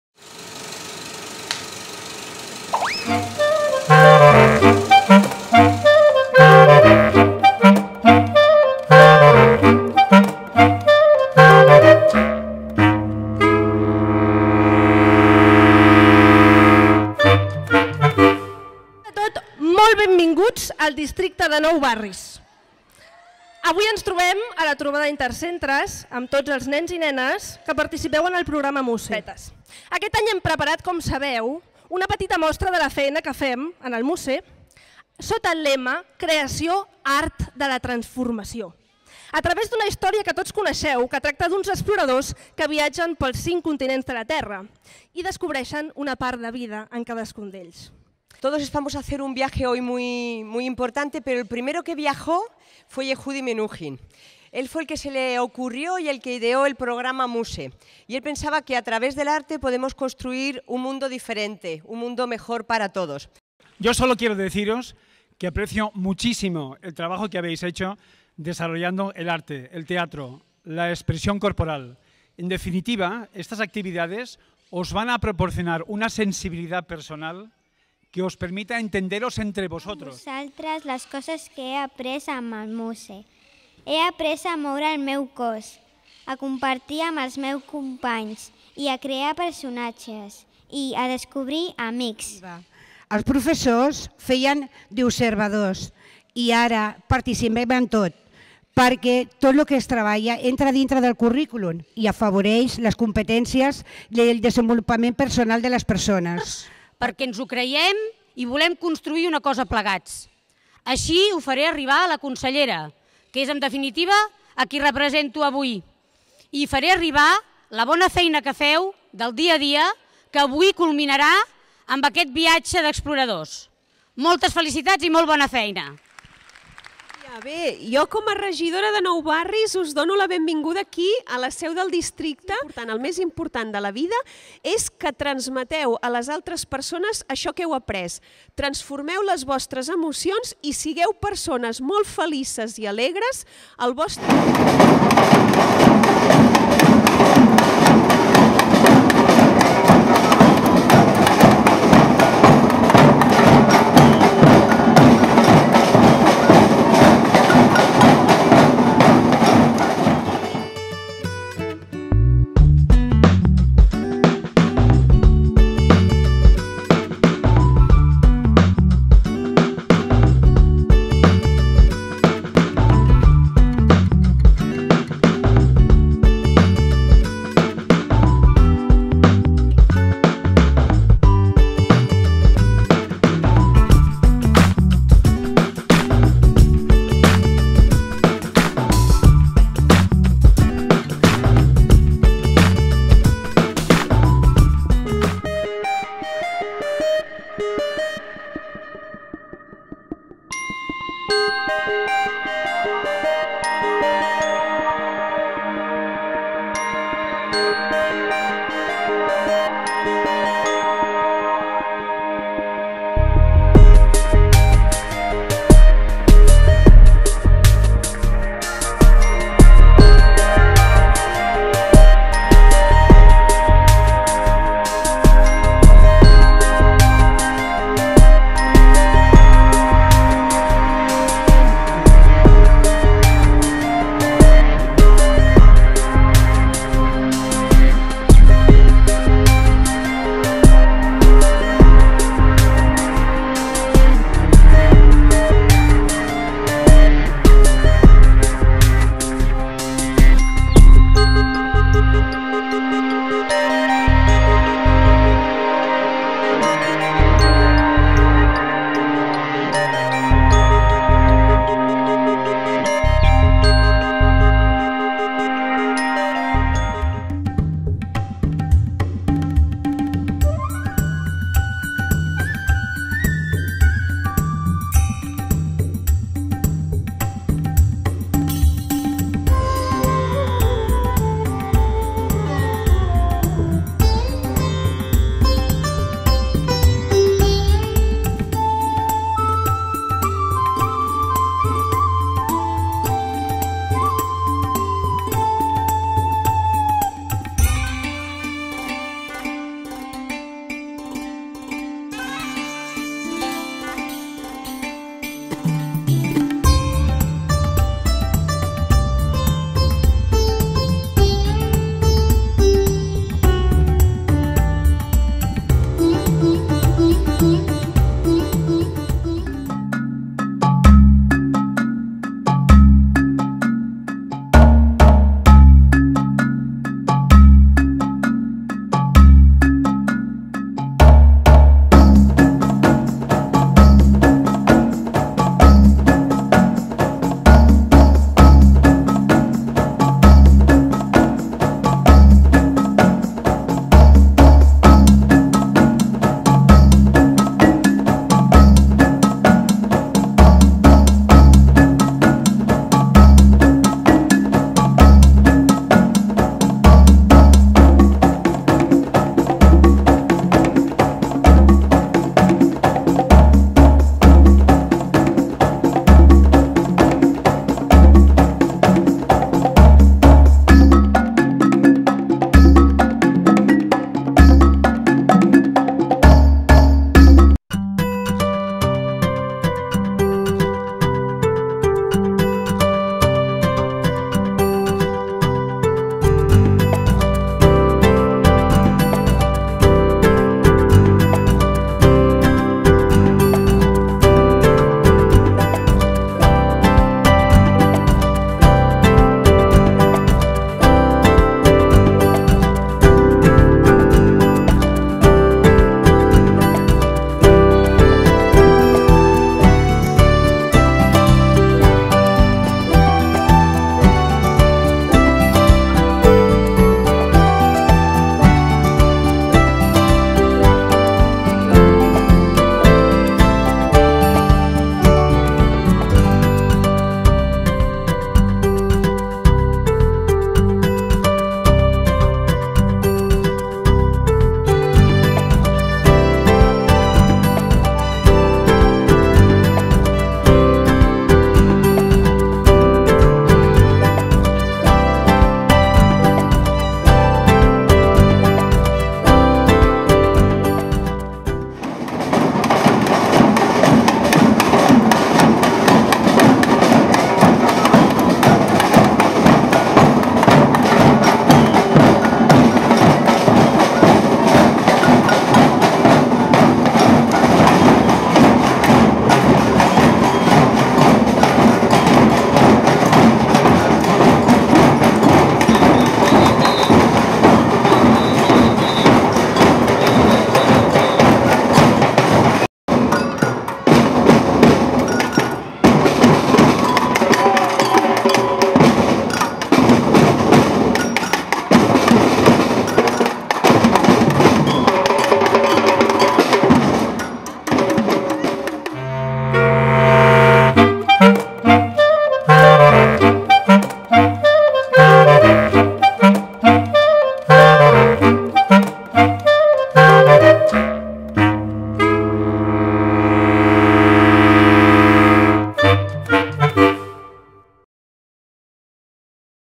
Trobada MUS-E Nou Barris
Encuentro Intercentros Mus-e Nou Barris, Barcelona, realizado en el curso escolar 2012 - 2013 por la Fundación Yehudi Menuhin España, en colaboración con el Distrito de Nou barris de la ciudad de Barcelona, con la participación de las escuelas.